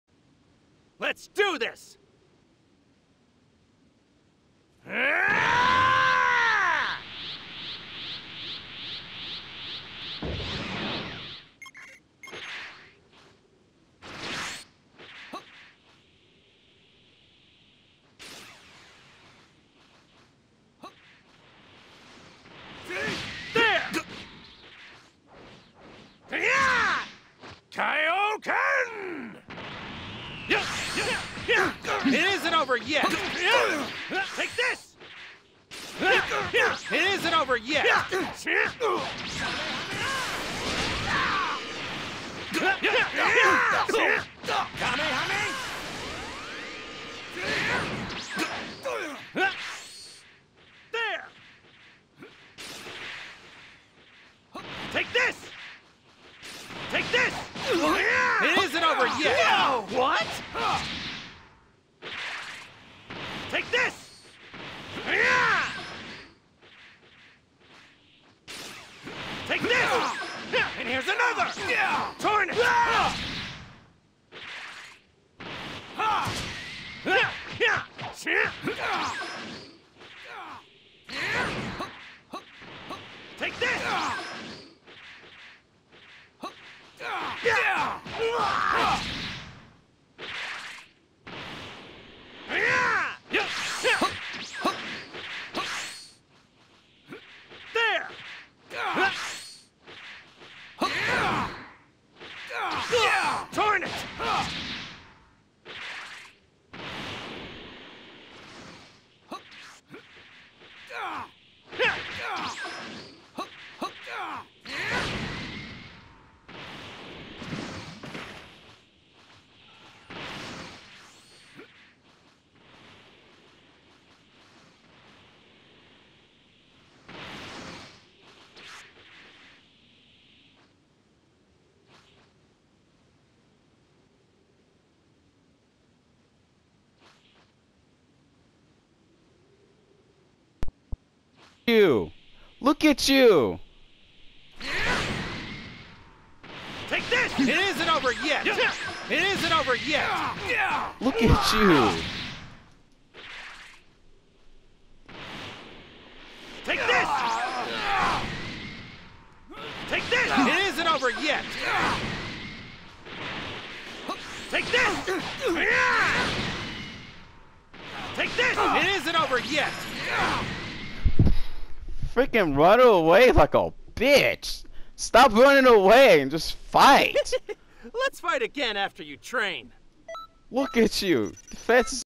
Grab spammer. Are you serious my dude. Stop playing like a goon. Fuck off with that spamming
The same dude who beat me before still spamming the grabs and runs away! Nah fuck that shit. I'm not letting this dude go without a punishment. You spamming the grabs cause I'm in kaioken! Nigga, the same shit he does is hella fucking stupid. Reddotheadshot, fuck off with that stupid shit.

This guy legit pissed me off after seeing the same shit he did before. I had to put my mic, telling him "look at you." Spamming the grabs and runs away is telling you that you suck like holy shit. You guys don't understand how upset that match was. I tried to make the match enjoyable ,but someone spam grabs and runs away, just pisses you guys off. That pissed me off so hard, I was done with this dude. I wanted to destroy him extremely hard for those amount of grabs. I hate when people plays like a bitch. It's so fucking ridiculous. Aaaahh. I got a headache.
I tried to make the video good for you guys. I just hate when people play like doodoo.
Fuck this dude. I'm going to work. See you guys!